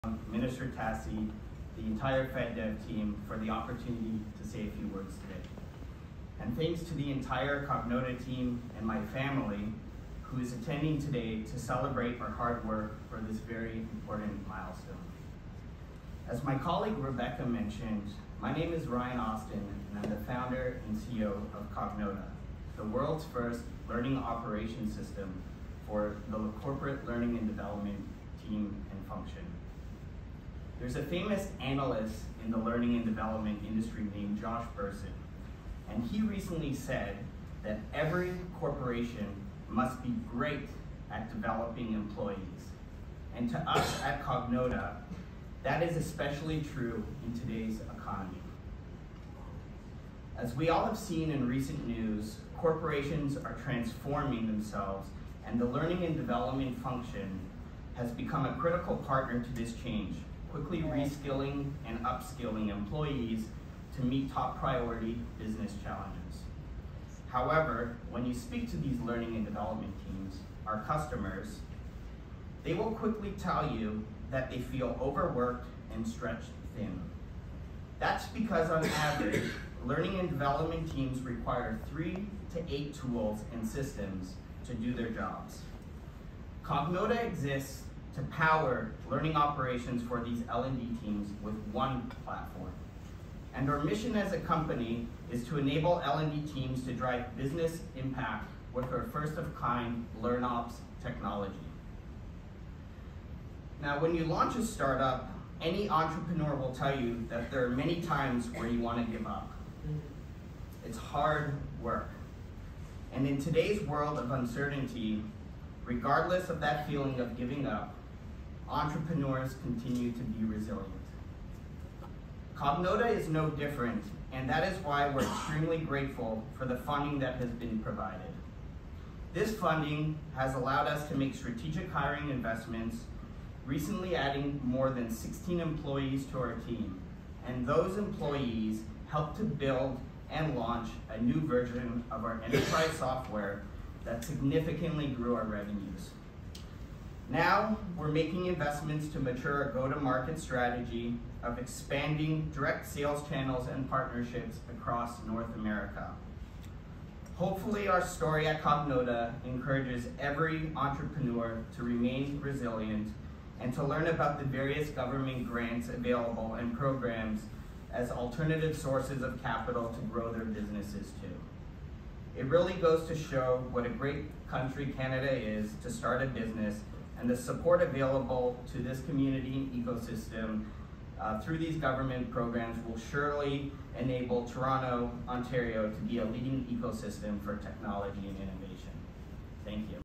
CEO of Cognota Accepts Funding from FedDev Ontario to Further Develop LearnOps® Technology
On Jan 10th 2023, it was announced that Cognota will receive over $1.9 million in funding from the Federal Economic Development Agency for Southern Ontario Jobs & Growth Fund.

Cognota CEO, Ryan Austin, spoke at the announcement event to share why this funding is important to Cognota. This funding will lead to the creation of new jobs in Ontario and the learning and development industry will benefit greatly as the company continues to invest in developing its first-of-its-kind LearnOps® technology.

To view the full press release made by the Government of Canada

If you'd like to learn more about what Cognota does